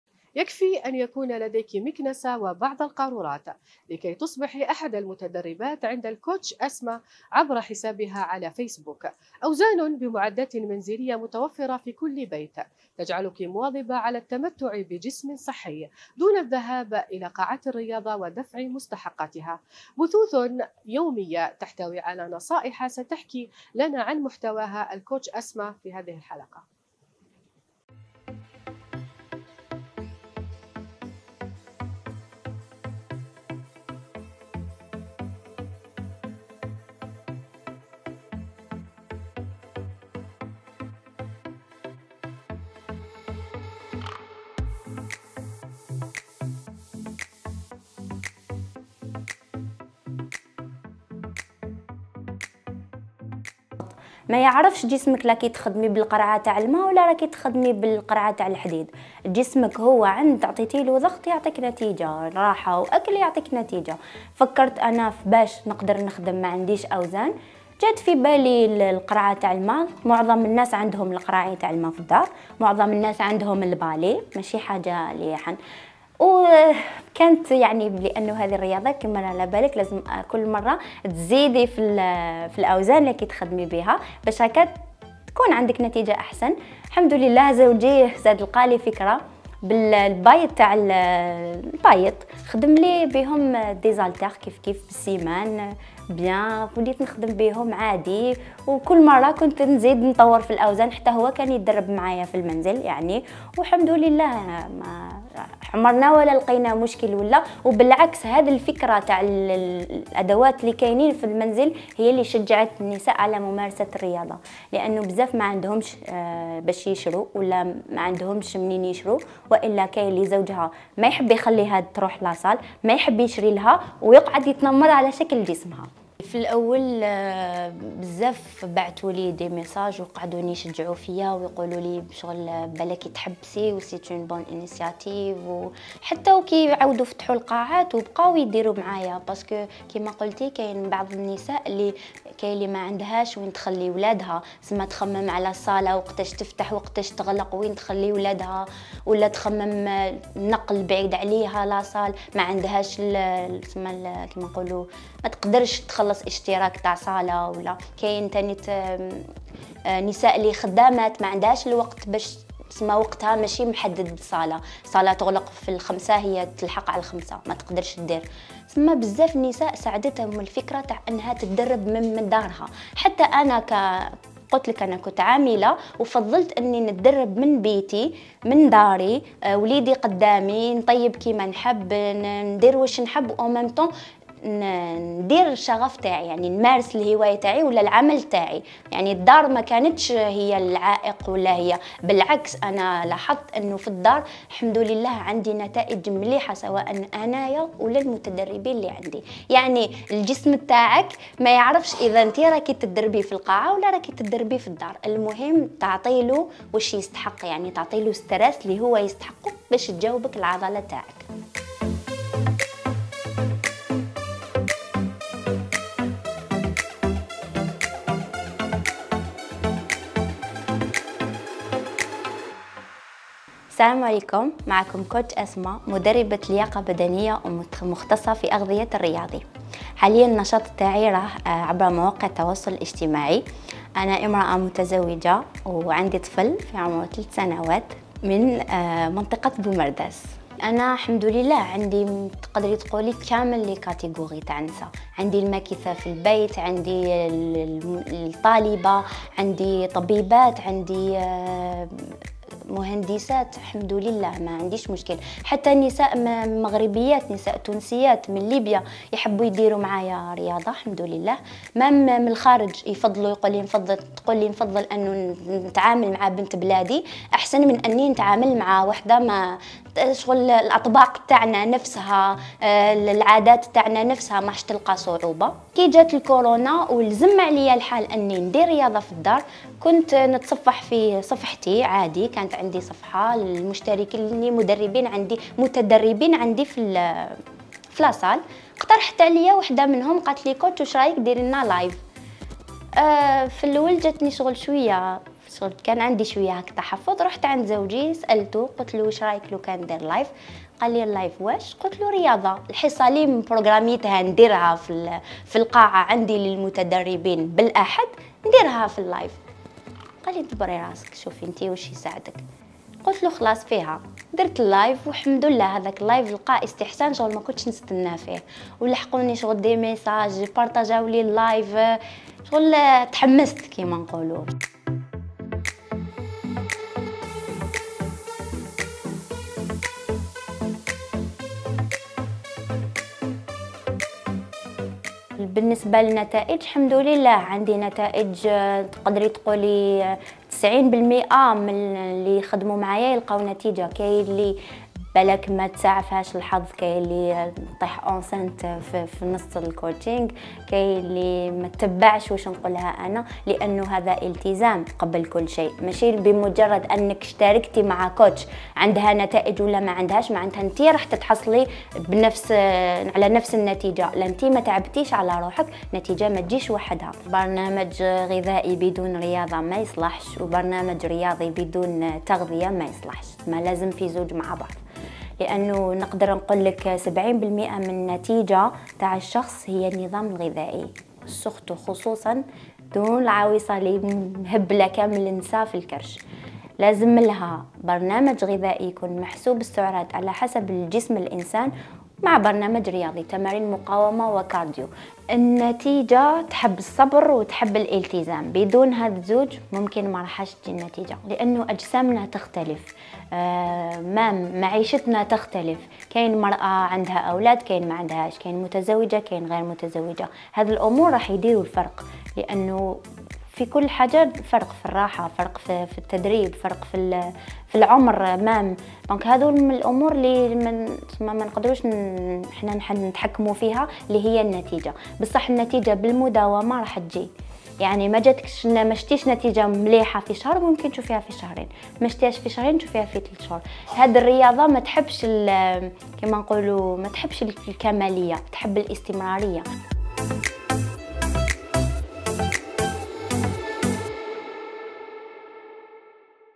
أسماء شابة تقدم حصص رياضية مجانية عبر "فيسبوك" باستعمال وسائل منزلية بسيطة 👏🥰
"يمكنك أن تتدرب معها بمكنسة وقارورة ماء".. أسماء شابة تقدم حصص رياضية مجانية عبر "فيسبوك" باستعمال وسائل منزلية بسيطة 👏🥰